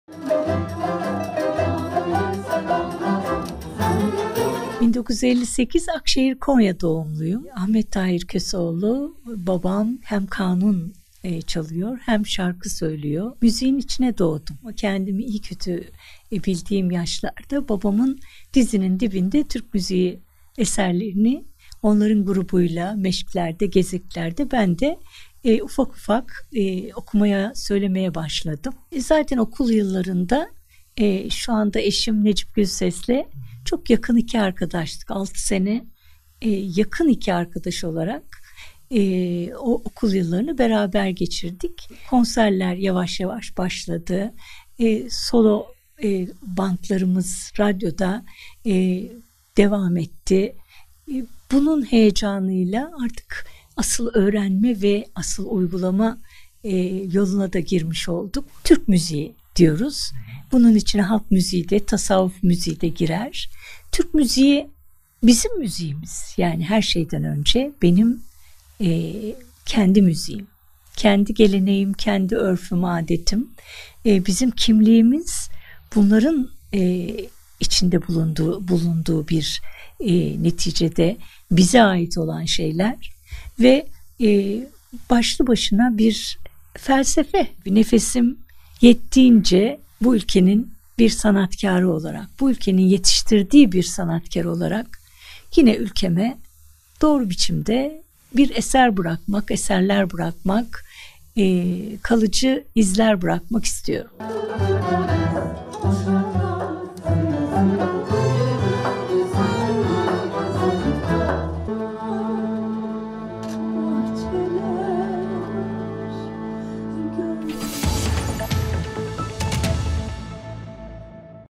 Melihat Gülses, Türk müziğinin dününü ve bugününü anlattı
Sanatçı Gülses, "Türk müziği hakikaten bizi anlatan, bizi söyleyen, kültürümüzün en önemli noktalarından biri. Gençlerin ilgisi yok filan denir ya hiç de öyle olduğunu düşünmüyorum. Gençler de çok ilgi gösteriyor, gayet meraklılar." dedi.